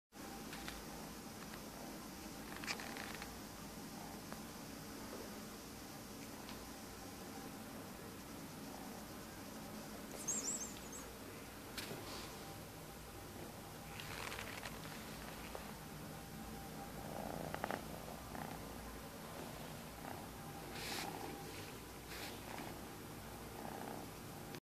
A cat and his string
All the siblings got adopted but this lil guy wasn't :( He was still at the shelter when I filmed this but he has since found his forever home.